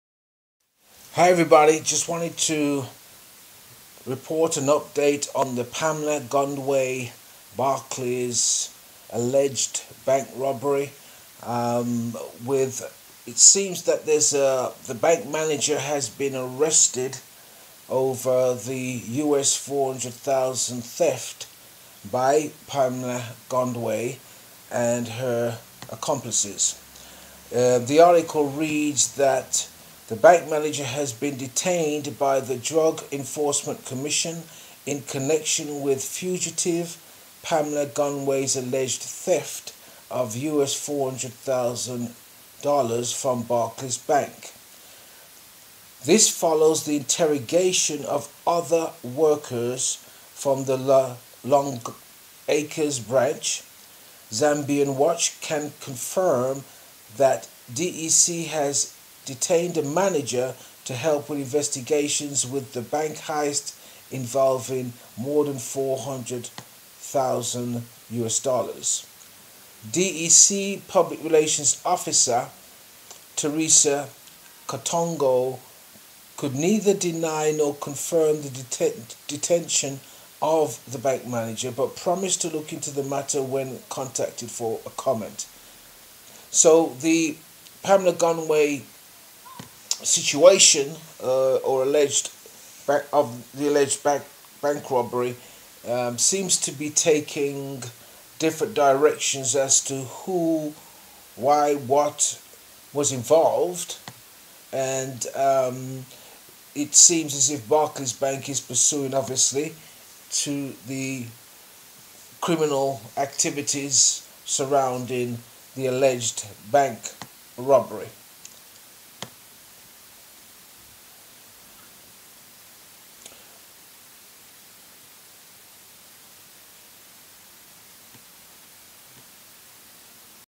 Pamela Gondwe Barclays Bank Theft Manager Arrested!
Pamela Gondwe Barclays Bank Theft Manager Arrested! The Manager of Barclays Bank at the time of 400,000 USD theft it is reported has now been arrested. I did a previous video report on Pamela Gondwe personal statement suggesting 'She was not alone' as the vault requires 3 keys held by 3 different people. What do you think of this alleged bank crime and why would the Bank Manager think he or she would ever escape such a crime? Thank you in advance.